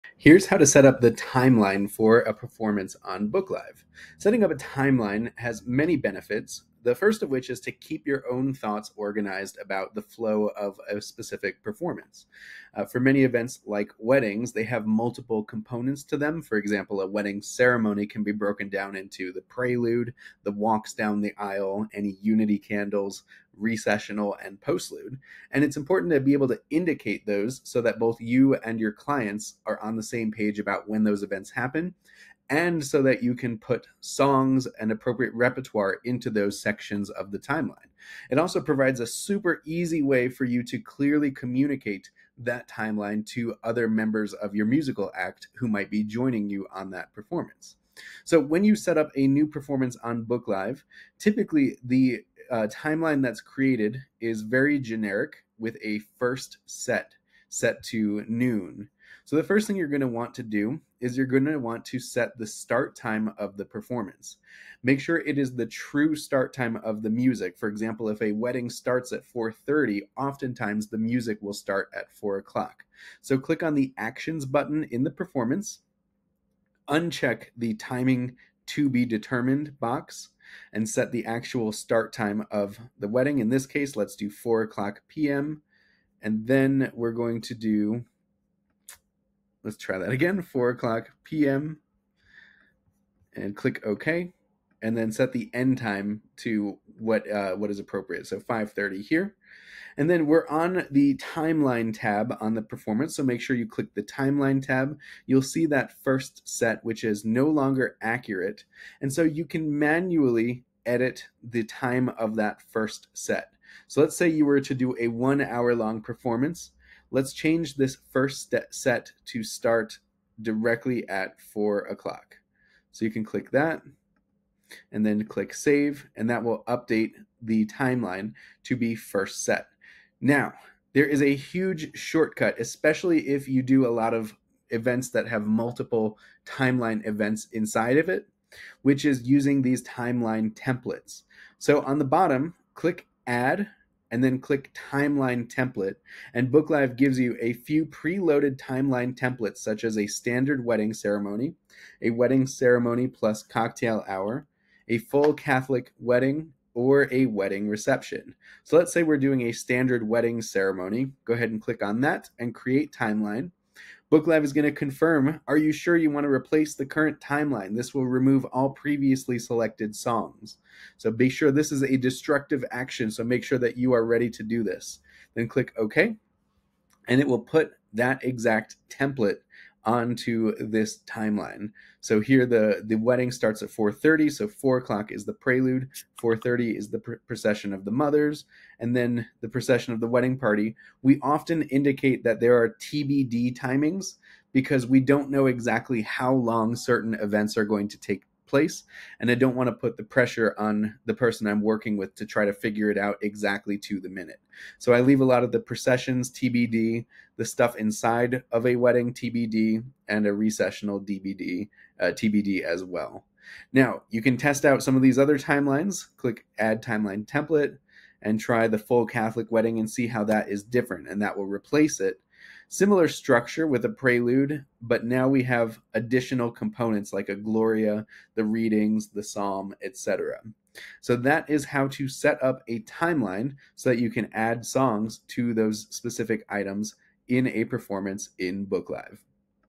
Setting Up Performance Timelines 🎶
Learn how to set up a timeline for your performance on BookLive. This feature helps you organize the flow of your event, ensuring clear communication with your clients and fellow musicians. Whether you're planning a wedding, corporate event, or any other performance, this tutorial will guide you through creating and customizing your timeline for a seamless experience.

Key Takeaways:

How to set the start time and end time for your performance.
Using timeline templates for quick setup.
Customizing the timeline to suit your event's needs.